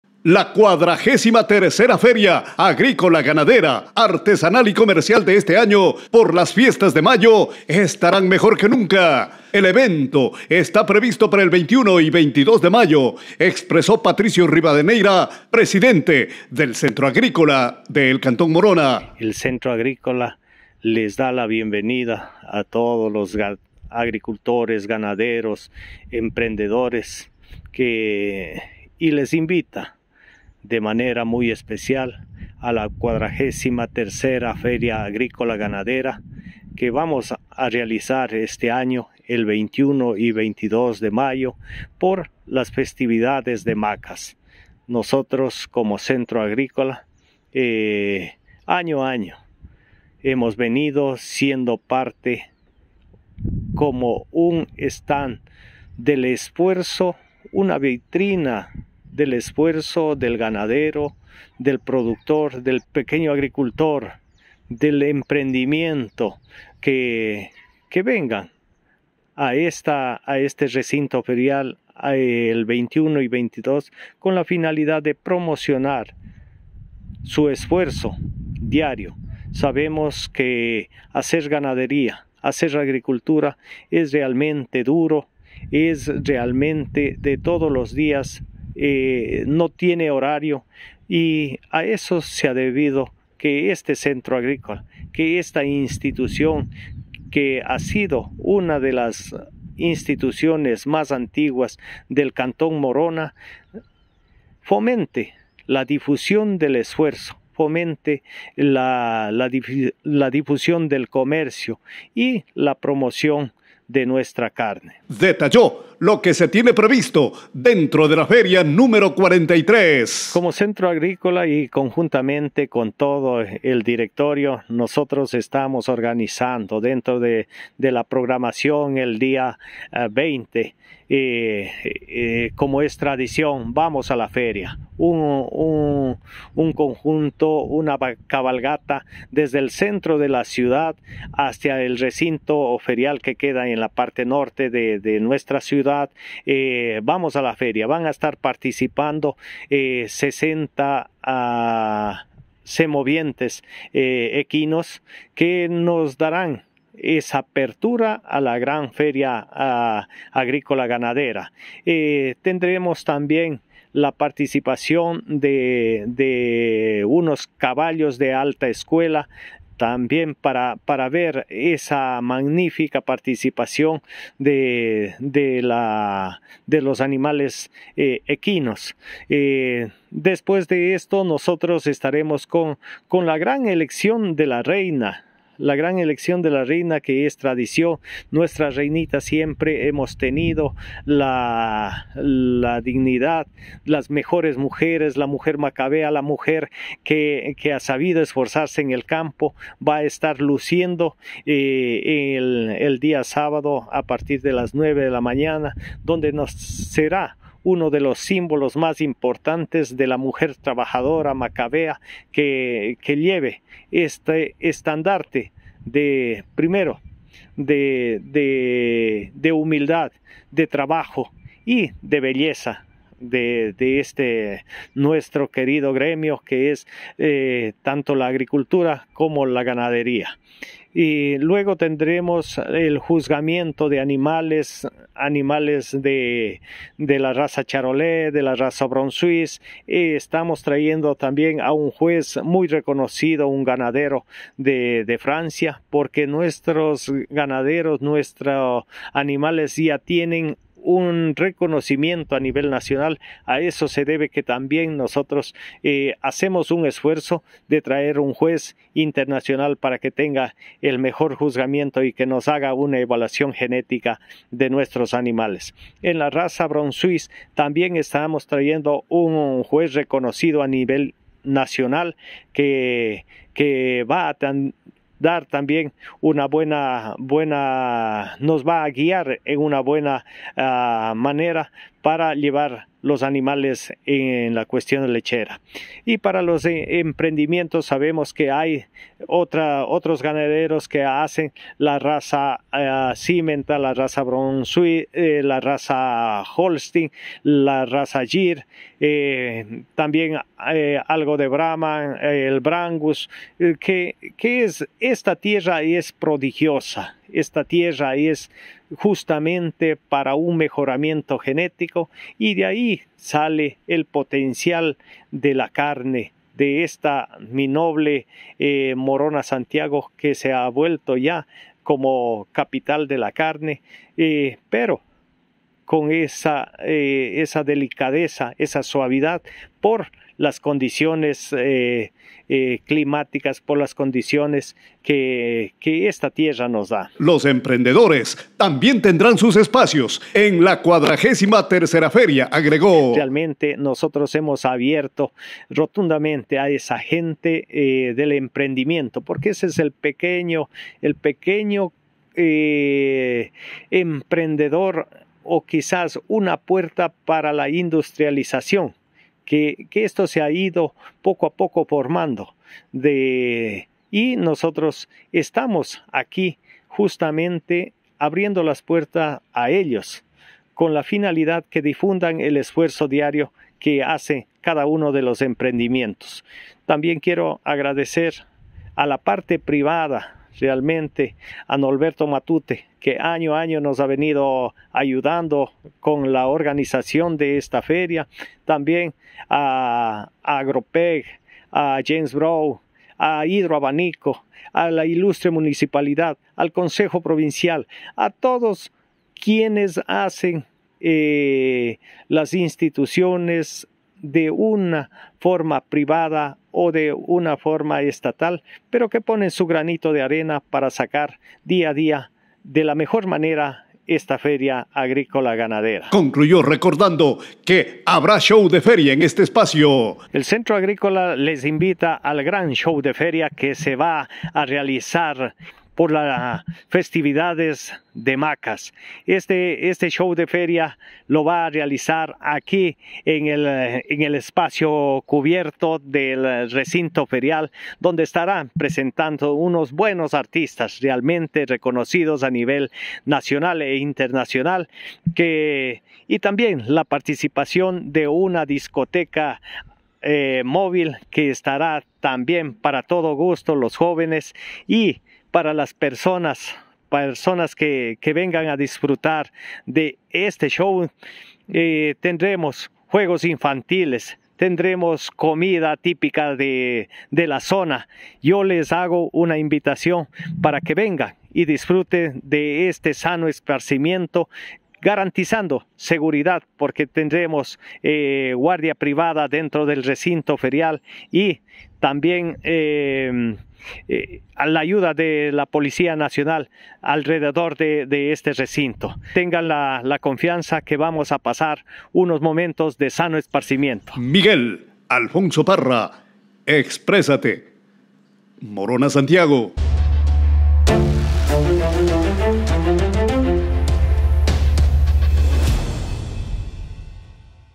El 20 y 21 de mayo, "XLIII Feria Artesanal, Turística, Agrícola, Ganadera y Comercial Macas 2023"
[INVITACIÓN] INTERESANTE: MoronaSantiago, se alista la Feria Agrícola Ganadera más grande de la provincia. La cita será en la ciudad de Macas el 20 y 21 de mayo.

Patricio Rivadeneira, Presidente del Centro Agrícola informa detalles sobre los preparativos. Están disponibles stands para impulso de marcas, empresas, turismo, artesanías, gastronomía. Además se realizará un gran show artístico.